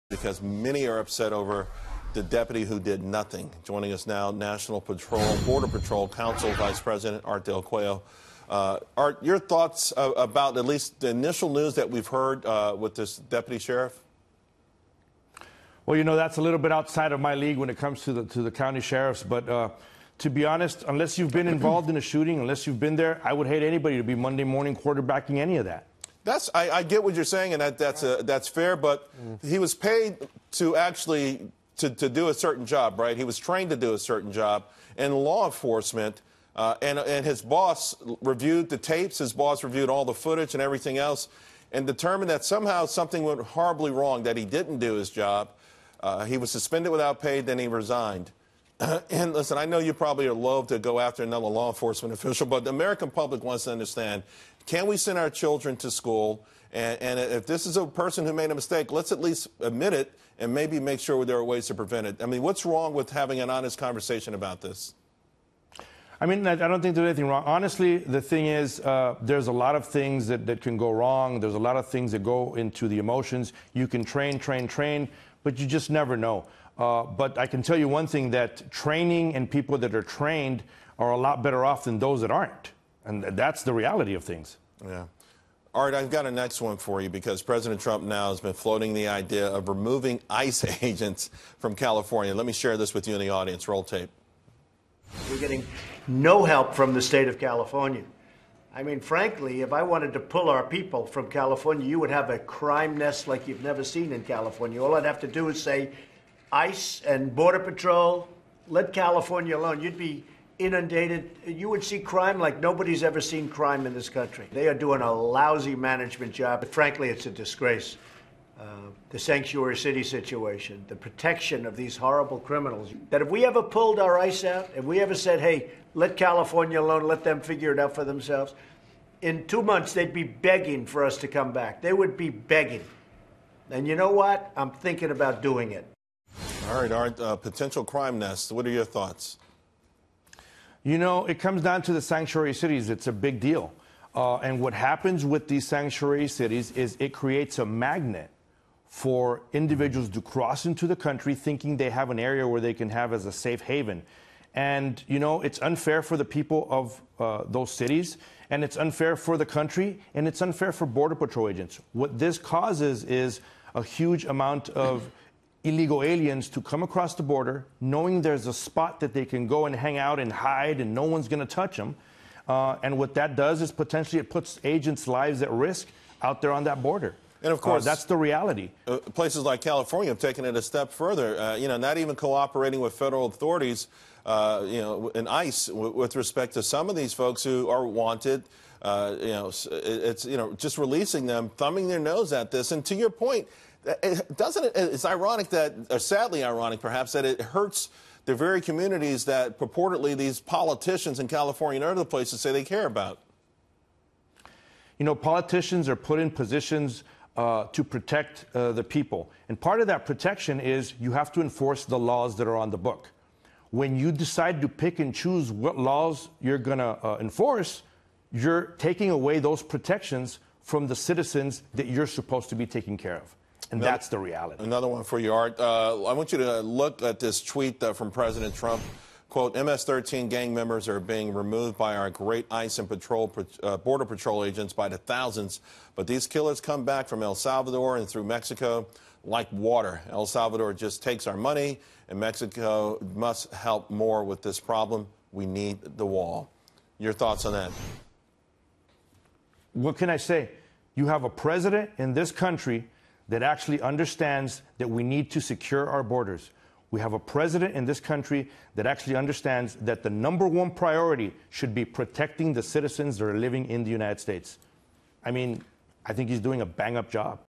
Trump threatens to pull ICE agents from California
National Border Patrol Council V.P. Art del Cueto on a deputy who didn't enter the Florida school during the shooting, President Trump threatening to pull ICE agents from California and efforts to build a wall along the border with Mexico.